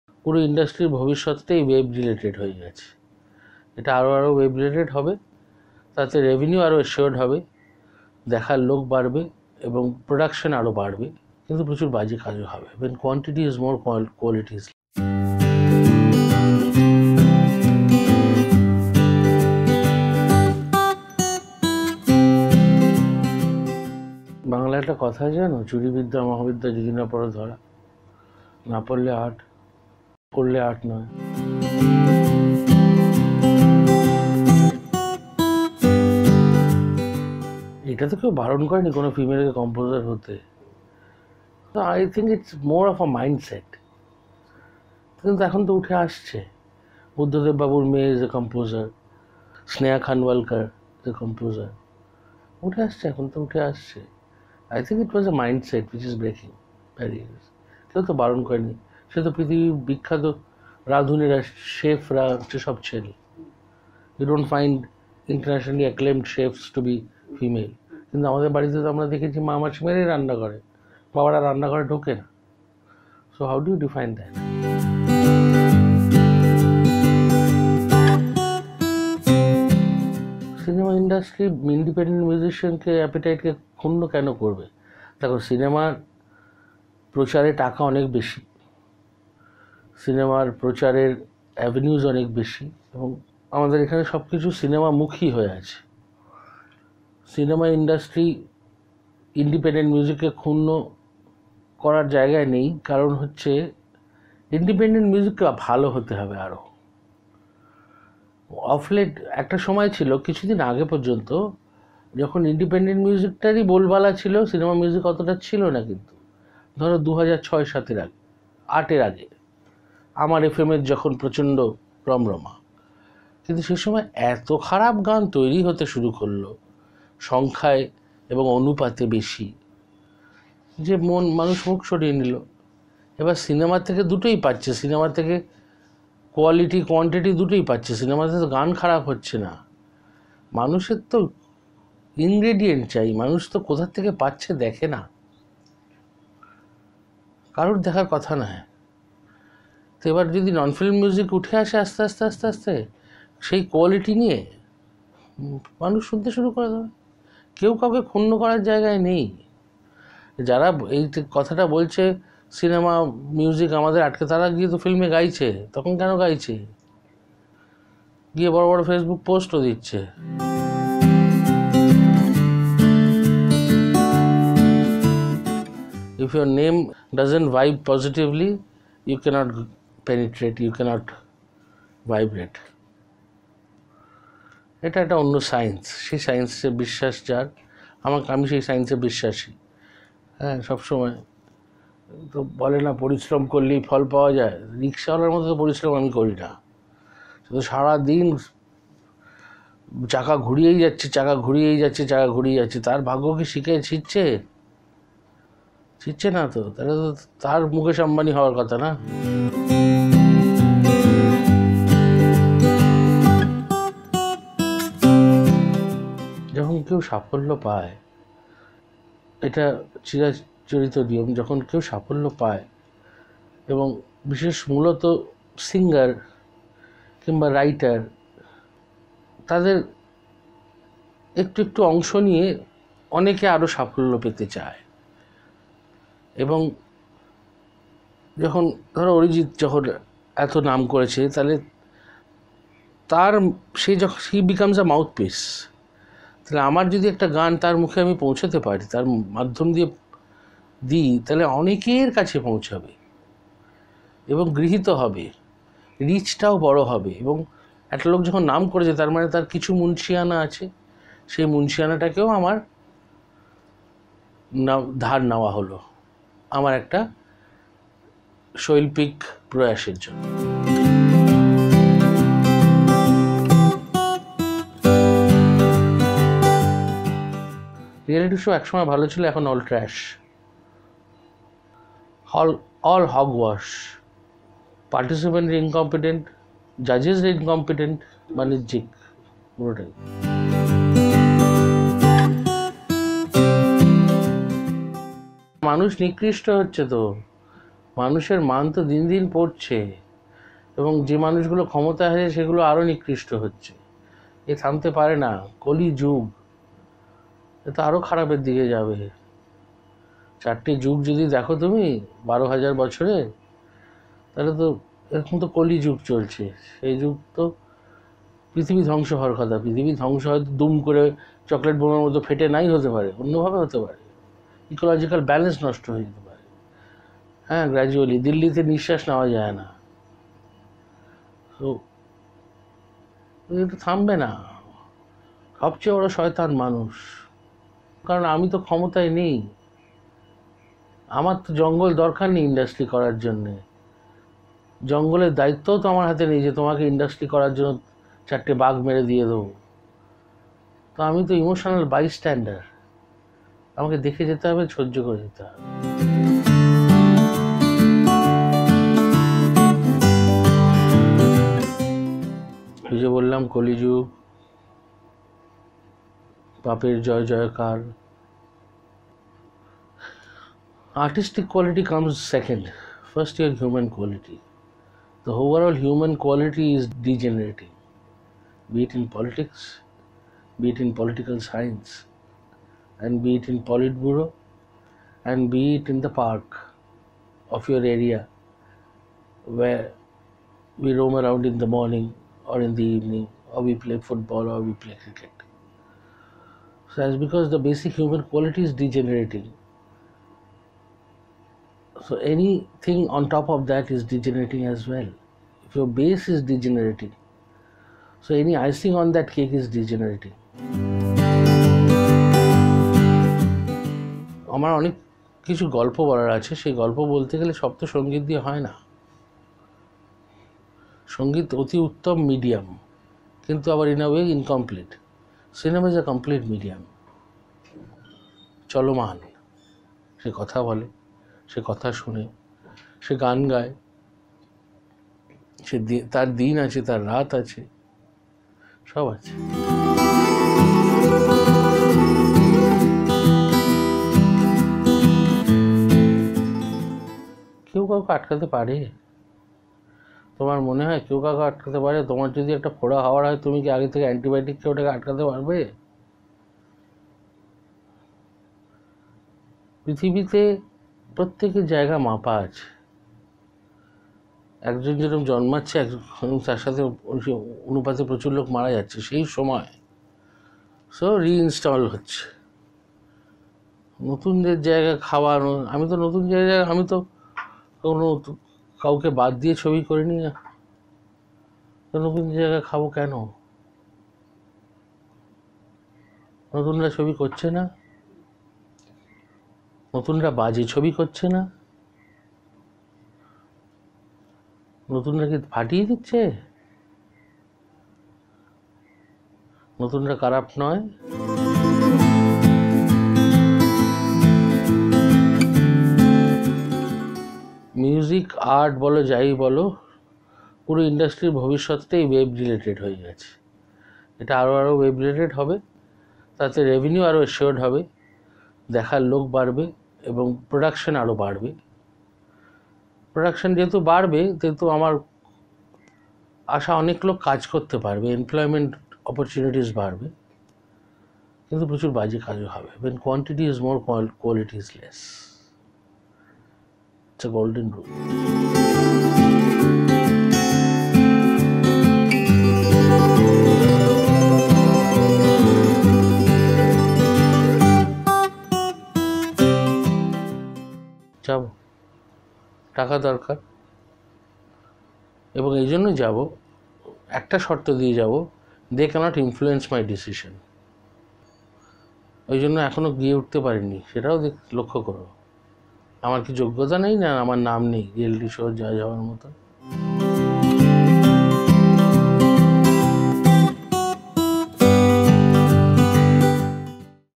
Indraadip Das Gupta talks about Reality Shows & Human Animal Conflict | Panorama Ep 14 | RoughSheet
He is one of the most versatile, renowned and respected music director working in Tollywood today. In this episode of Panorama, IndraadipDasGupta, the composer for films such as Chaplin, Mishawr Rahasya and Hawa Bodol, talks about singer monopoly, lack of female composers and the future of BanglaMusic industry.

0:20 - Is Plagiarism an Art?
0:32 - Lack of Female Composers
1:15 - Does Cinema subjugate Independent Music?
2:51 - Chiradeep to Indraadip
3:30 - 'Monopoly' of a Singer
4:31 - Reality Shows
4:50 - Human Animal Conflict
5:57 - Politics in Music
6:51 - From Music Director to Director
7:21 - A Hindrance to Newcomers?
8:21 - Future of Music Industry
8:59 - Time Frame with Indraadip

Videography Team:

Souvik Sarkar
Angshuman Mandal
Ritodeep Mukherjee
Pulasta Chakrabarty